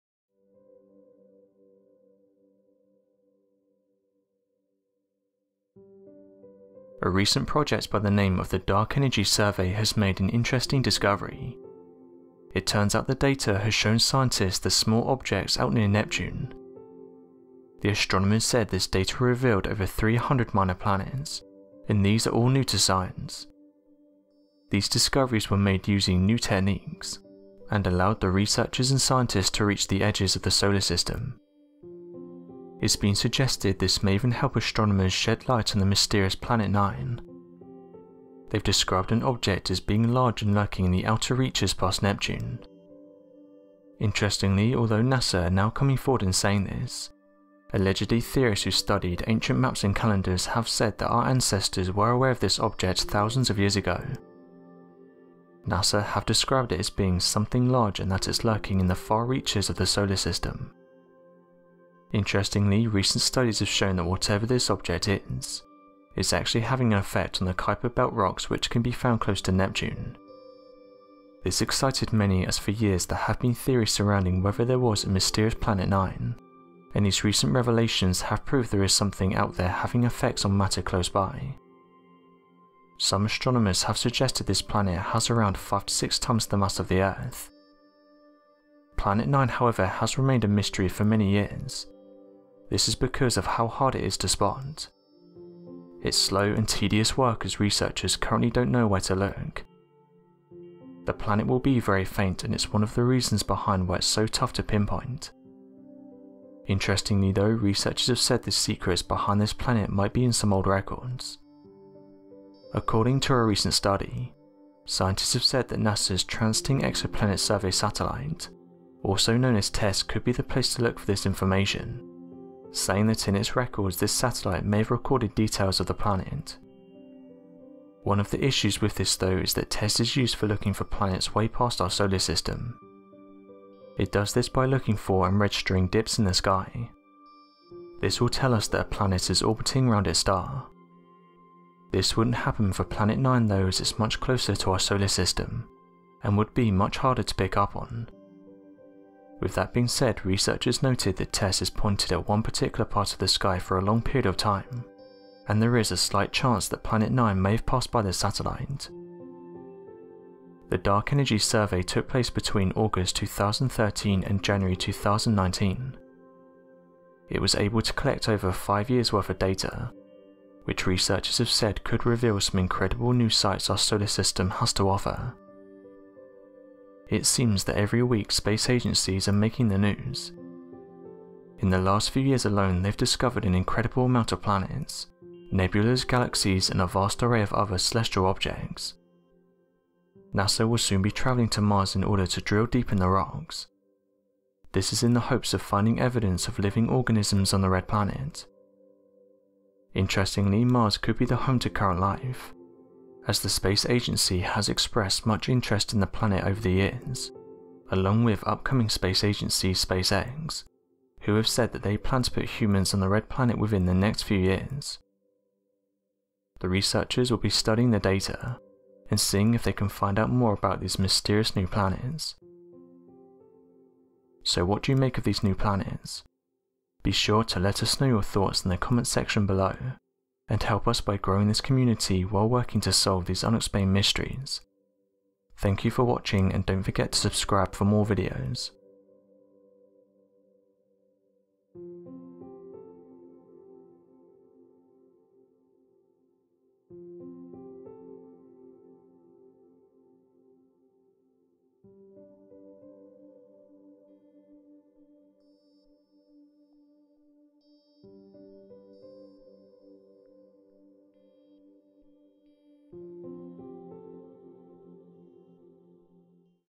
Astronomers Have Just Discovered Over 100 Minor Planets At The Edge Of Our Solar System
Astronomers have just discovered over 100 minor planets at the edge of the Solar System. Today, we take a look at these new 100 minor planets that have just been discovered.

A recent project by the name of the Dark Energy Survey has recent made an interesting discovery. It turns out the data has showed scientists that there's small objects out near Neptune.

The astronomers said that this data revealed over 300 minor planets, and these are all new to science. These discoveries were made using new techniques and allowed the researchers and scientists to reach the edges of the solar system.